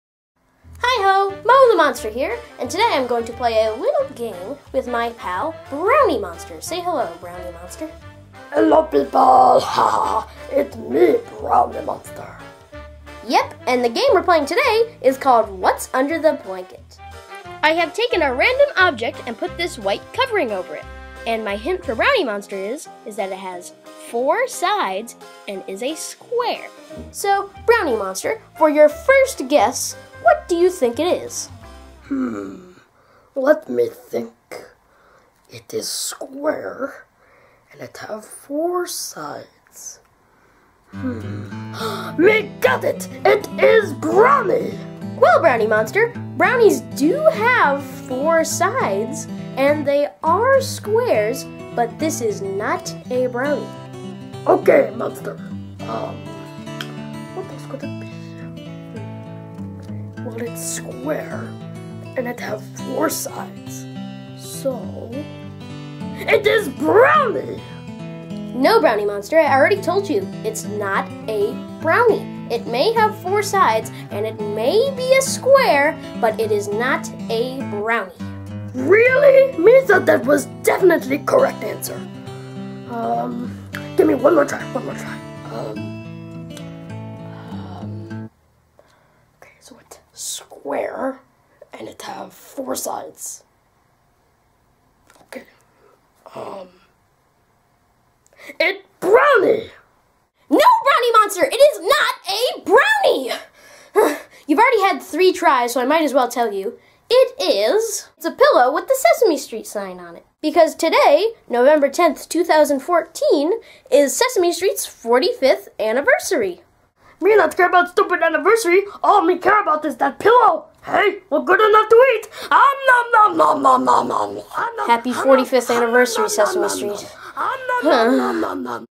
Happy 45th Anniversary Sesame Street!
Mo and Brownie Monster spoof a classic Sesame Street Skit!